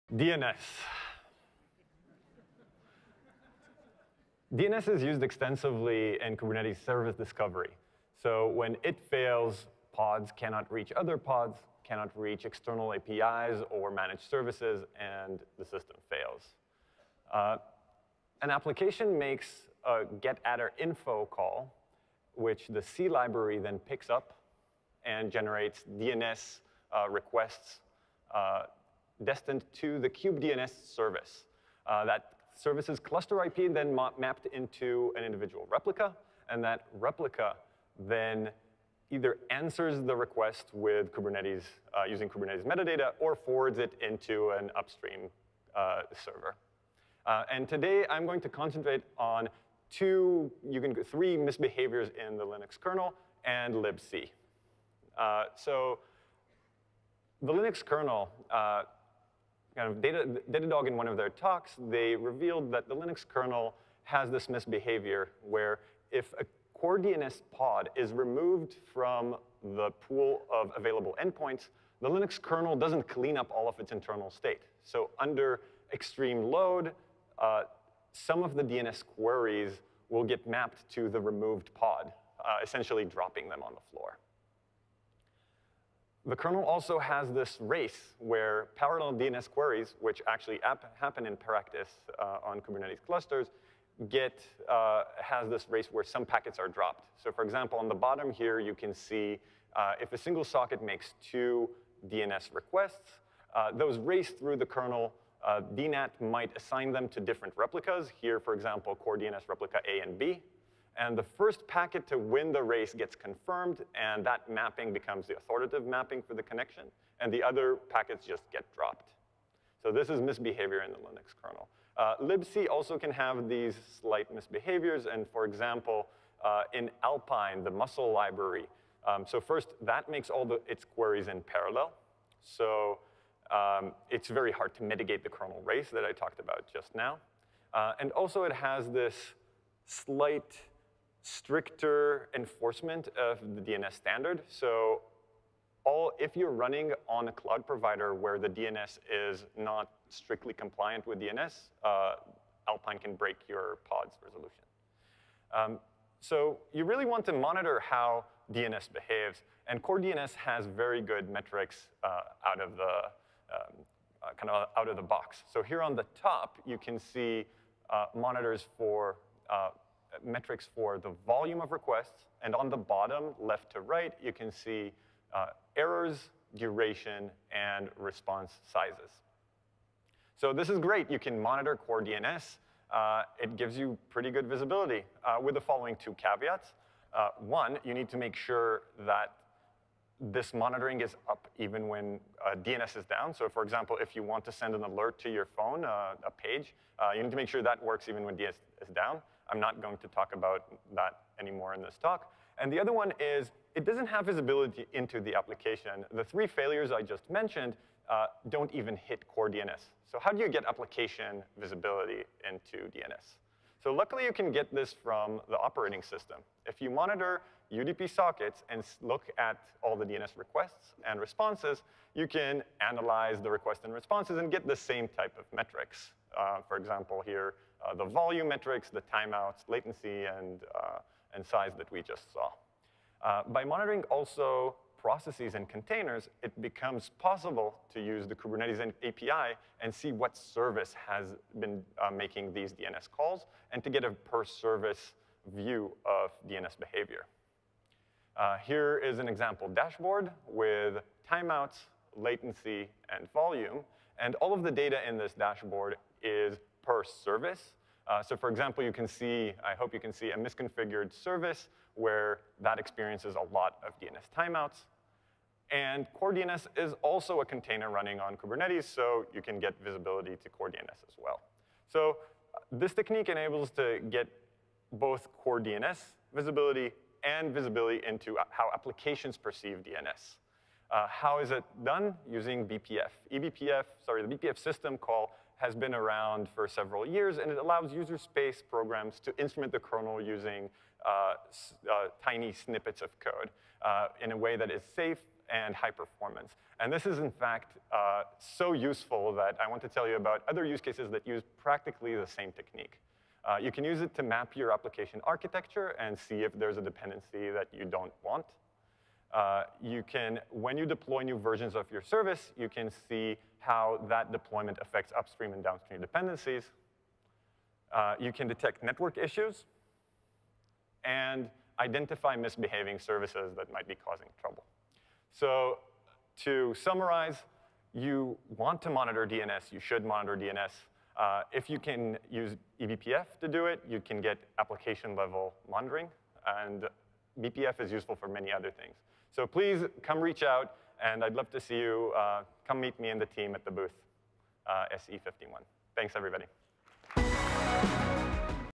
Lightning Talk: Is Your Kubernetes Cluster's DNS Working? - Jonathan Perry, Flowmill
Your Kubernetes cluster is gaining traction and more and more developers are bringing up new services. That’s great news. But you’ve been getting reports of intermittent service failures that you haven’t been able to track down. It doesn’t seem to be anything else...could it be DNS? Maybe we’re not running enough DNS pods or they are restarting too frequently?  This talk will explain how to actually measure DNS health for your Kubernetes cluster and properly plan its capacity. We will share some specific mechanisms to gather DNS traffic information per service both with some standard Linux tools and systematically with eBPF.